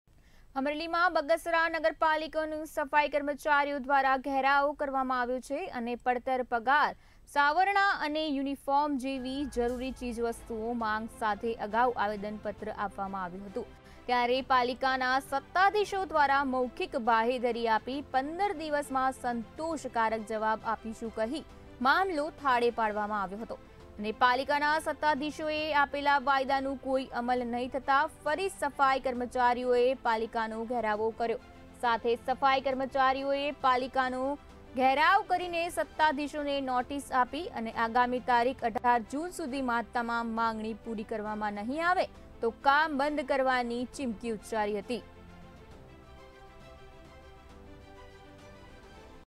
સફાઇ કર્મચારીઓએ કર્યો નગરપાલિકાનો ઘેરાવ
સફાઇ કર્મચારીઓએ કર્યો નગરપાલિકાનો ઘેરાવ , Sanitation workers surrounded the municipality
અમરેલીમાં બગસરા નગરપાલિકાનો સફાઈ કર્મચારીઓ દ્વારા ઘેરાવ
પડતર પ્રશ્નો અને જરૂરી ચીજવસ્તુઓની માંગ સાથે નપાનો ઘેરાવ
પડતર પગાર, સાવરણા અને યુનિફોર્મ જેવી જરૂરી ચીજવસ્તુઓની માંગ
15 દિવસમાં માંગનો નિરાકરણ લાવવા સત્તાધીશોએ આપી હતી બાહેંધરી
તા. 18 જૂન સુધીમાં માંગણી સ્વીકારવામાં નહિં આવે તો કામ બંધ કરવાની ચીમકી